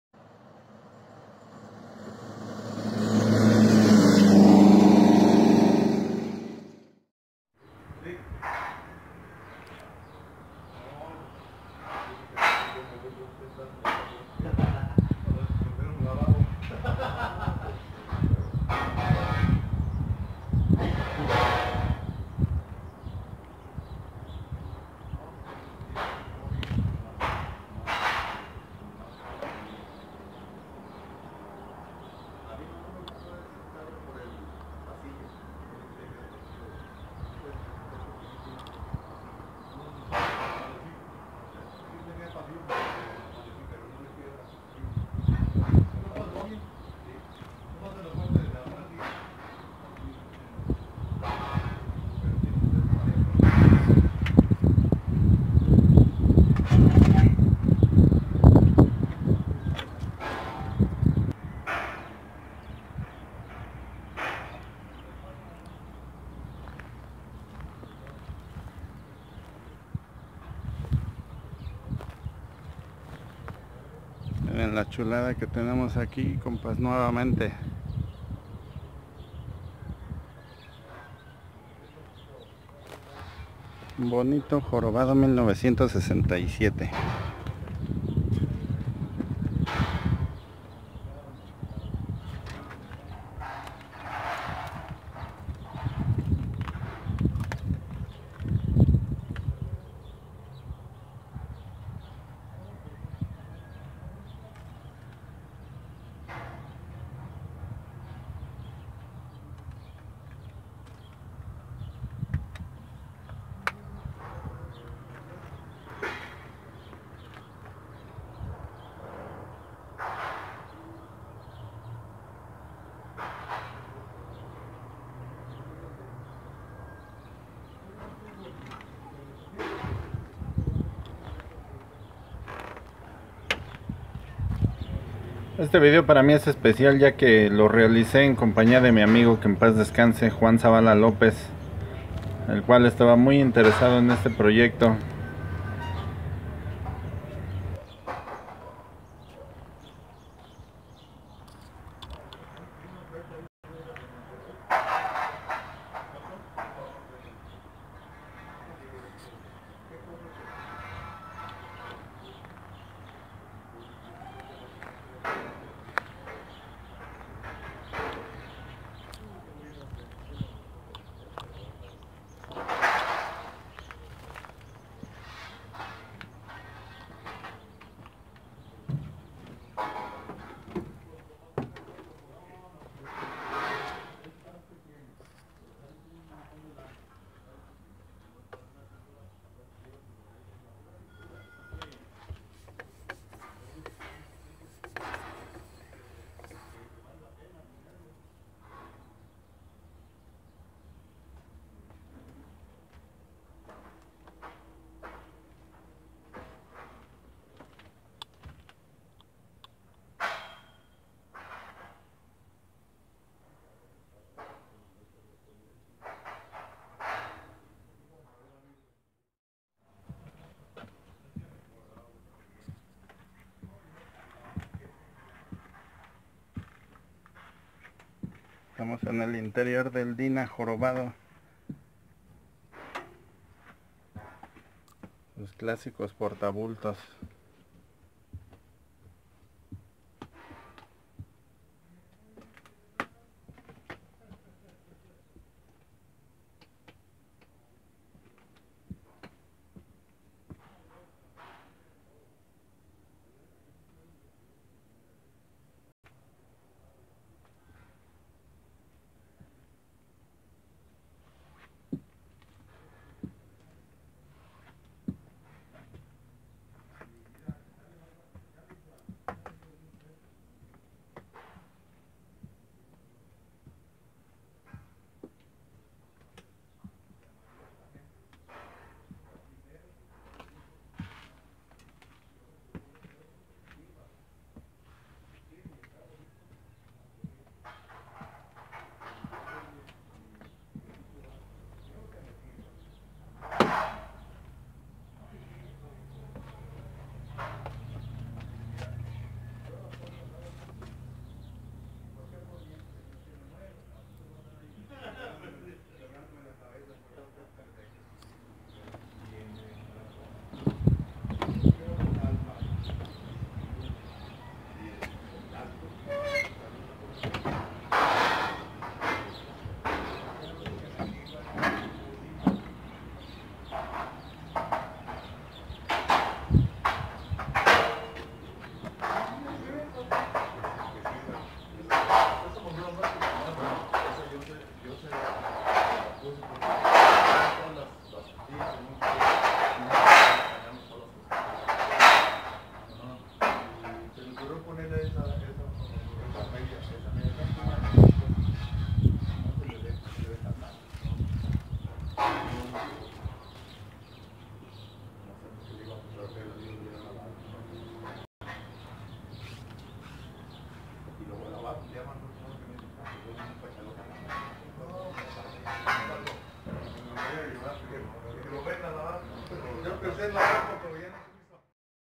DINA 1967 Se seguirá pudriendo ahí ?
Saludos compas y aficionadas a estos Autobuses.   les dejo este video de una visita rapida a Guadalajara